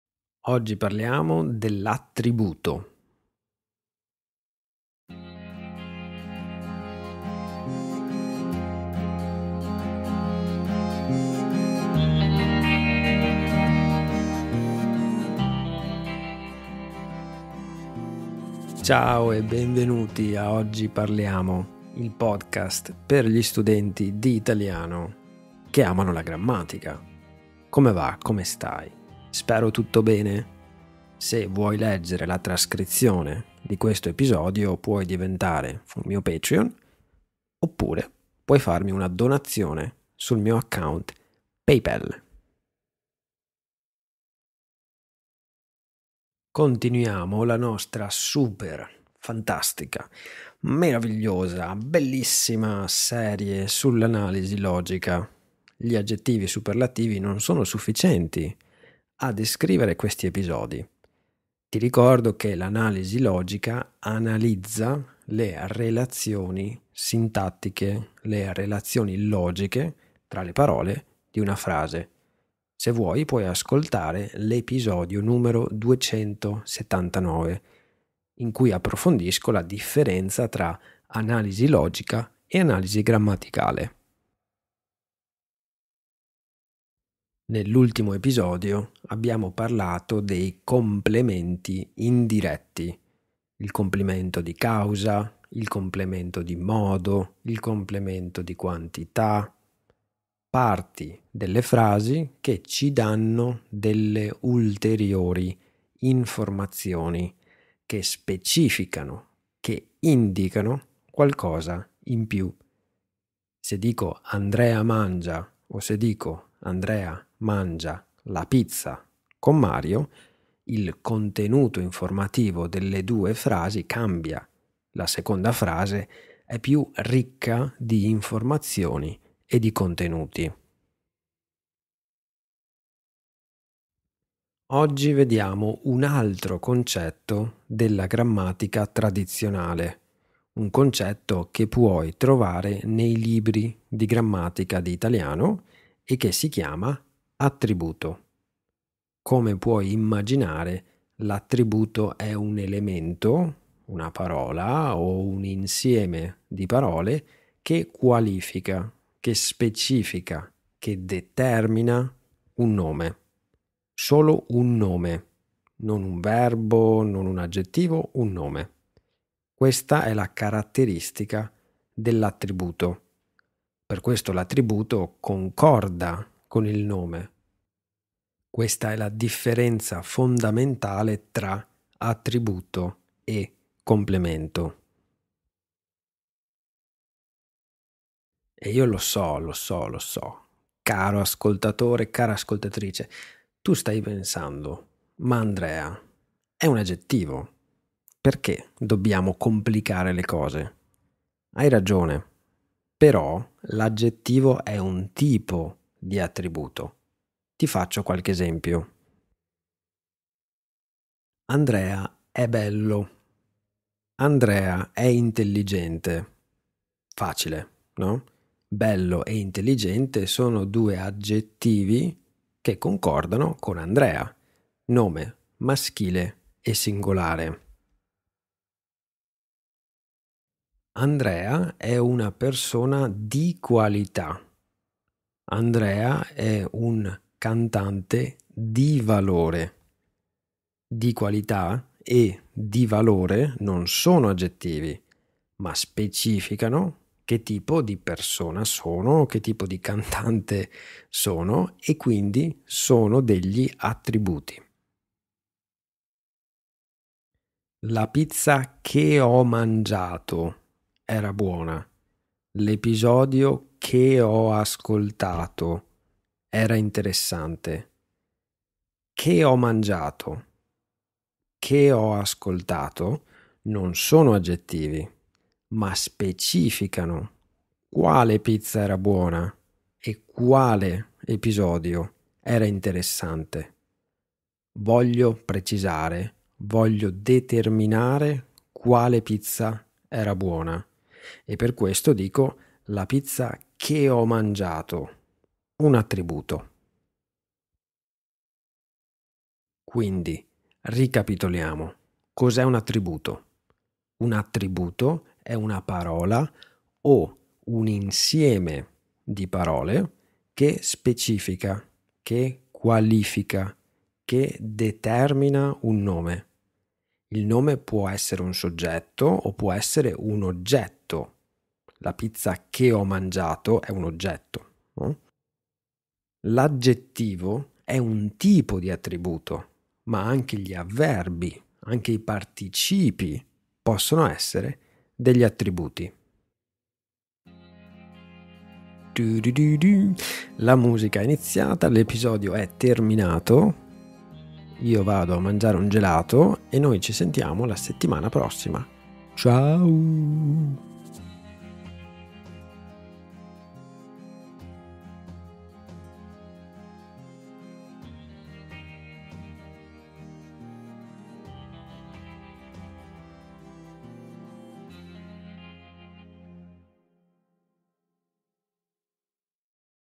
303. GRAMMATICA: L'attributo
Nell’episodio di oggi cerchiamo di capire insieme cos'è un attributo: è un aggettivo? è un avverbio? è un complemento? (spoiler: sí e no)

Contenuti
⏱️ 02:12 Cos'è un attributo?
⏱️ 03:06 L'attributo è un aggettivo?
⏱️ 04:58 Definizione

🙏🏼 Grazie ai miei nuovi Patreons Milena, Jim e Tim e a Vinnie e Sarah per le loro donazioni su PayPal.